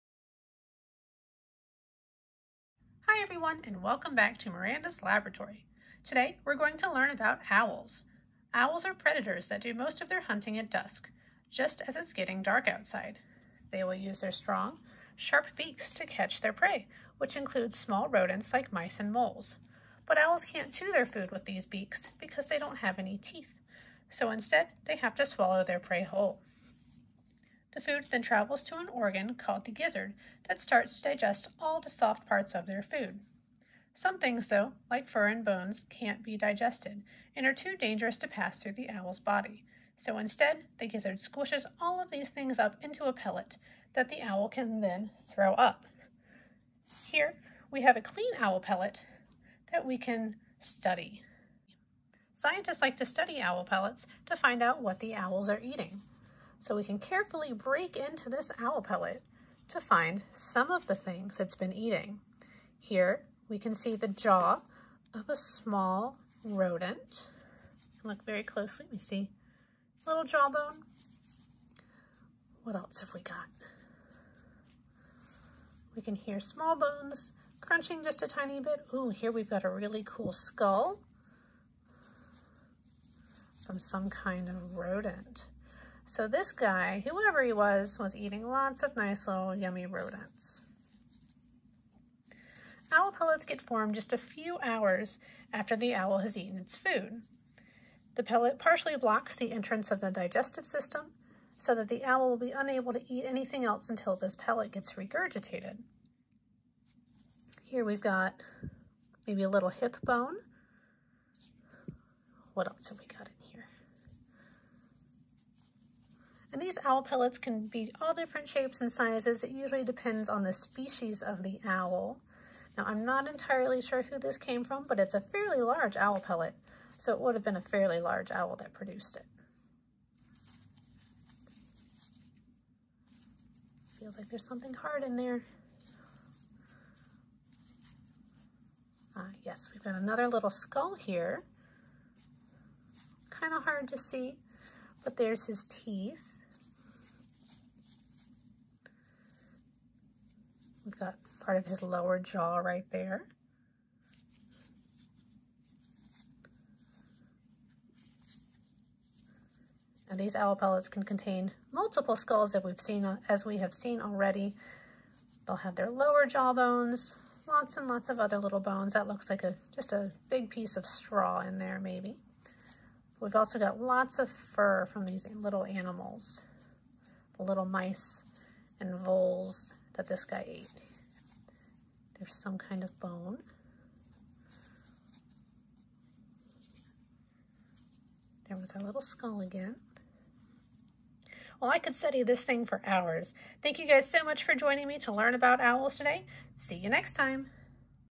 Nature Experience: Owl Pellet Dissection
What do owls eat for dinner? Find out with educator Miranda as she dissects an owl pellet. WHO knows what we’ll discover!